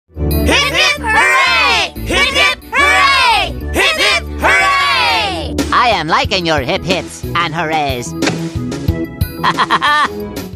My Reaction That Caillou Got Cancelled Back In 2021
Finally That Bratty Kid Is Off The Air

Note:ToddWorld Is Still My New Favorite Show As Of 2022 Also Known As Last Year